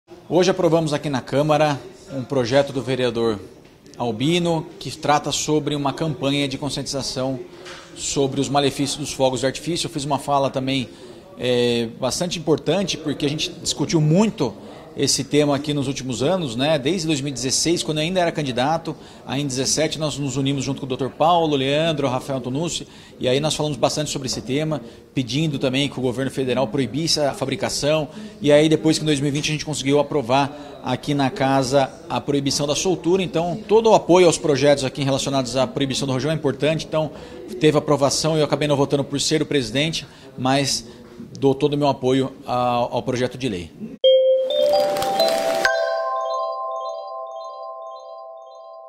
PROJETO DE LEI No. 12969/2019 em 09.02.2021
PROJETO DE LEI No. 12969/2019 - ANTONIO CARLOS ALBINO - Institui a “CAMPANHA DE CONSCIENTIZAÇÃO SOBRE OS MALEFÍCIOS CAUSADOS PELO USO DE FOGOS DE ARTIFÍCIO COM EFEITOS SONOROS” (estampido).